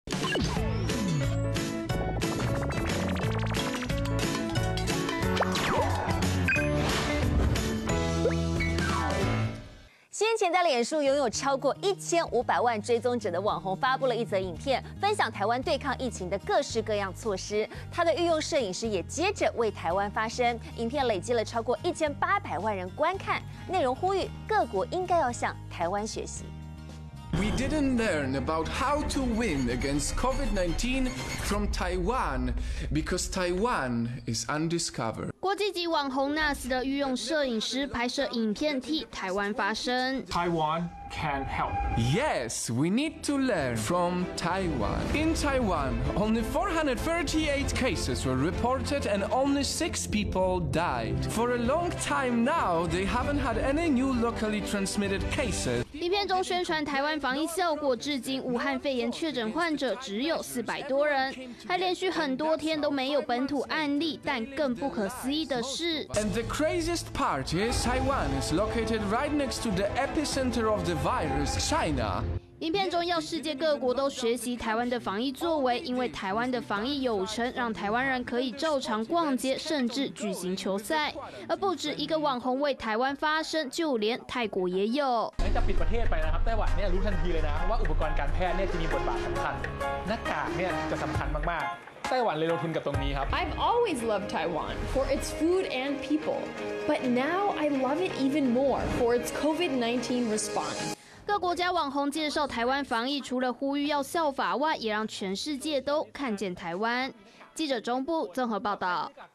16台灣戰疫揚名！ 國際網紅發聲挺 破1800萬觀看20200516【年代小確幸】PART16
圓山再掀話題 超殺方案999住一晚
台北圓山飯店為了鼓舞國人，先前在零確診的日子點燈為國人加油，創意手法吸引許多民眾來打卡拍照。雖然是老字號飯店，但圓山近年來行銷手法越來越新，最近還推出史無前例的破天荒優惠，只要吃自助餐，就能用999元入住飯店。

北台灣新景點 萬里八千坪"繡球花園"
隨著疫情稍稍趨緩，很多民眾在周休假期，會選擇出門透透氣，而今天要跟大家介紹一個，全新的旅遊景點，新北萬里的繡球花園，佔地約八千坪，平均比陽明山大上七倍左右，園主透露，整個花況最盛開的時間，將會落在五月中下旬，但別忘了，雖然是戶外景點，還是要做好相關措施，因應防疫新生活。

減少久戴口罩不適感 減壓帶.面膜商機延伸
從疫情開始到現在，民眾戴口罩上街至少也已經有三個月。長期戴口罩讓許多民眾皮膚或是耳朵都有不舒服的感覺。有業者就從這裡看到商機，開發口罩減壓帶、口罩專用面膜，讓民眾戴口罩也能減輕不適感。

吸入"旺旺水神"噴霧 恐傷眼.呼吸道黏膜
旺旺水神抗菌液，爭議不斷，有民眾發現旺旺旗下的麵包店，入口處會擺放霧化器，不只進出會接觸到，食物也會沾染，醫師說，次氯酸水拿來飲用、接觸皮膚都可能有傷害，更何況吸入噴霧，一不小心，會造成呼吸道黏膜損傷。

公所發"旺旺水神" 男怨標示不明誤喝 "就像消毒水"
水神抗菌液使用保特瓶包裝，乍看就像是礦泉水，沒警語、沒標示，導致有民眾誤喝 緊急送醫，我們獨家訪問到這名男子，他是在四月底收到鄉公所發的水神，喝一大口後驚覺味道不對，到急診吊點滴，照了Ｘ光，所幸沒有大礙，也沒有引發過敏，但事發至今，相關單位卻沒有任何的回應或是道歉！

鹹的甜的通通能夾 創意蔥油餅吸人潮
疫情趨緩，大家在做好防疫的狀況下，開始可以走出家裡了，畢竟台灣的美食真的不少，像是常見的蔥抓餅，也能加入創意，把厚切沙朗漢堡排乾煎後，和九層塔夾進餅裡，口感就像披薩。或是把乳酪絲、起司片、起司粉混合，搭上九層塔蛋，吃起來奶香濃郁。而最特別的是甜口味，蔥油餅用乾烙的方式，淋上巧克力醬搭配鳳梨片。

這蛋餅有30公分 外酥內香口味多變
不只有蔥抓餅變化，蛋餅也是，有業者把麵皮和蛋煎到兩面金黃，加進滿滿餡料再捲起來，外型像是捲餅，又充滿蛋香，特別的是長度竟超過30公分，直接拿起來咬相當過癮。最受歡迎的口味是鮪魚蘋果，喜歡重口味的可以吃美墨辣牛，香辣香辣很涮嘴。

明星開店不容"疫" 推優惠.隔離包廂吸客
不少餐廳都受到疫情衝擊，包括名人開的店，藝人劭昕的火鍋店，觀察到客人需求，緊急加工，隔出獨立包廂保持安全距離，唐從聖的雞排店，則是推出優惠，吸引老客戶回流。

藍心湄副業不敵疫情 KiKi餐廳旗艦店6月中暫歇業
藝人藍心湄經營的KiKi餐廳，受疫情重創，宣布復興旗艦店下月16日起暫時停止營業。KiKi旗艦店達150坪，在各店中面積最大、租金最貴，主攻觀光客，但最近觀光客銳減。

直擊現況! 澳門政府發3萬嘸效 熱門景點人潮稀落
澳門經濟主要仰賴觀光，受到疫情影響，觀光客進不來，過去知名的大三巴現在非常冷清，伴手禮街有三分之二都拉下鐵門，賭場生意更是暴跌九成以上，為了刺激消費，澳門政府發放居民每人八千元澳門幣，相當於台幣約三萬，想藉此提振經濟。

香奈兒恐將漲價! 南韓湧搶購潮業績狂飆
疫情也影響到精品產業，法國香奈兒總公司宣布漲價，最大漲幅高達百分之17到25，各國市場跟進的風聲不斷，南韓的百貨公司，因此湧現排隊搶購潮，甚至有人凌晨就到場排隊，不少精品業者業績提升至少三成。

稱全程戴罩被抓包 KARA奎利改口"脫口罩20秒"
南韓疫情期間，藝人照跑夜店，先前爆發梨泰院群聚感染，女星朴奎利也在場，事後他發表聲明道歉，強調全程戴口罩，但後來又改口，入場20秒沒戴，引起一片罵聲，另外還有男藝人宋旻浩，到夜店演唱，在場者口罩全戴在下巴上。

防疫小教室 動手做美味龜苓膏
為了慶祝零確診，中央疫情指揮中心準備了龜苓膏來慶祝，也讓龜苓膏掀起一波討論。我們走訪迪化街的中藥行老店，來帶大家了解龜苓膏的小知識，也要教你怎麼做出好吃的龜苓膏。

中藥治武肺? "中西合併"最短8天驗無病毒?
中藥也可以治療武漢肺炎嗎! 台灣研究團隊沿用SARS治療經驗，找出獨門中藥配方，號稱可以在八到十天內清除患者體內病毒目前正在爭取納入疫情指揮中心治療指引，未來也將跟藥廠合作，希望可以為治療帶來一線曙光!

歐美兒童罹"川崎氏病" 專家研究是否與武肺有關
歐美武漢肺炎確診的兒童，竟然併發罕見的"川崎氏症"! 專家認為，也許跟"體質"有關，因為川崎氏症也是一種血管壁發炎，冠狀病毒可能引發各種發炎反應，當然也包含血管發炎，不過，在亞洲其他國家甚至國內，沒有看到併發川崎氏症的武漢肺炎兒童個案，指揮中心認為可能需要更多資料佐證，不論如何，這也是個值得全球警覺的警訊!

台灣戰疫揚名! 國際網紅發聲挺 破1800萬觀看
台灣防疫成效全球有目共睹！先前在臉書擁有超過一千五百萬追蹤者的網紅，發布一則影片，分享台灣對抗疫情的各項措施，他的御用攝影師也接著為台灣發聲，影片累積超過1800萬人觀看，內容呼籲各國需要向台灣學習。

疫情趨緩! "不可能任務"等大片復拍倒數
電影《不可能的任務7》原定在義大利拍攝，因為疫情緊急喊卡，但隨著疫情趨緩，阿湯哥堅持回到原地拍攝，希望協助振興經濟，另外在紐西蘭拍攝的電影《阿凡達2》及《魔戒》的影集，由於紐西蘭政府防疫成功，即將重新復工。

下載【年代電視】APP：
iOS平台：